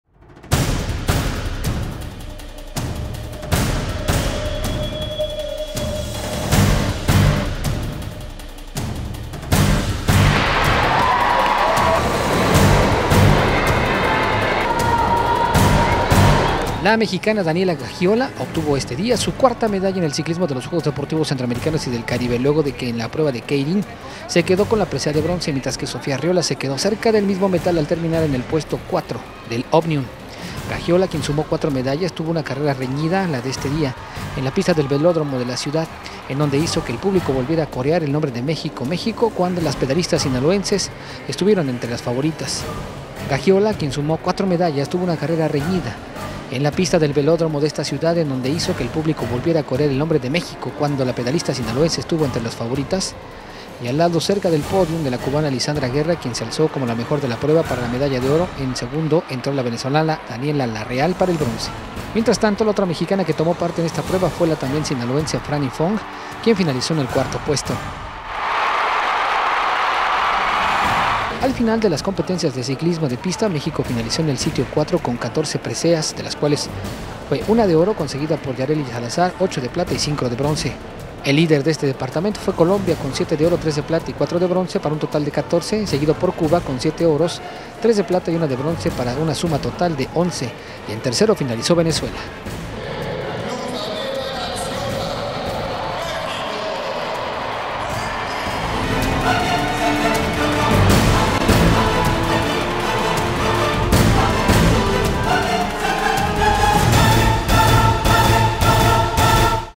Daniela Gaxiola Cierra con Bronce en Keirin de JCC
La mexicana Daniela Gaxiola obtuvo este viernes su cuarta medalla en el ciclismo de los Juegos Deportivos Centroamericanos y del Caribe Veracruz 2014, luego que en la prueba de Keirin se quedó con la presea de bronce; mientras que Sofía Arreola se quedó cerca del mismo metal al terminar en el puesto cuatro del Omnium.

Gaxiola, quien sumó cuatro medallas, tuvo una carrera reñida la de este viernes en la pista del velódromo de esta ciudad, en donde hizo que el público volviera a corear el nombre de México, cuando la pedalistas sinaloense estuvo entre las favoritas.

La mexicana estuvo en el podio a lado de la cubana Lissandra Guerra, quien se alzó como la mejor de la prueba, para la medalla de oro, en segundo entró la venezolana Daniela Larreal para el bronce.

Mientras que la otra mexicana que tomó parte en esta prueba, fue la también sinaloense Frany Fong, quien finalizó en el cuarto puesto.

Gaxiola comentó que la prueba de este viernes fue difícil sobre todo por la presencia de la medallista mundial, Lissandra Guerra, quien fue la ganadora de oro, pero también de la venezolana Daniela Larreal, quien tiene mucha experiencia en el ciclismo de pista.

Por su parte, la regiomontana Sofía Arreola se quedó a un paso del bronce en la prueba de Omnium al terminar en el sitio cuatro con una suma de 184 unidades, en donde la ganadora fue la cubana Marlies Mejías con 240, la plata fue para la colombiana Jannie Salcedo con 212 y el bronce fue para la venezolana Angie González con 203 puntos.

AL final de la competencias de ciclismo de pista, México finalizó en el sitio cuatro con 14 medallas de las cuales una fue de oro conseguida por Yarley Salazar, ocho de plata y cinco de bronce.

El líder de este departamento fue Colombia con 7 de oro, tres de plata y cuatro de bronce para un total de 14, seguido de Cuba con siete de oro, tres de plata y una de bronce para una suma de 11 y en tercero finalizó Venezuela con dos de oro, cuatro de plata y seis de bronce.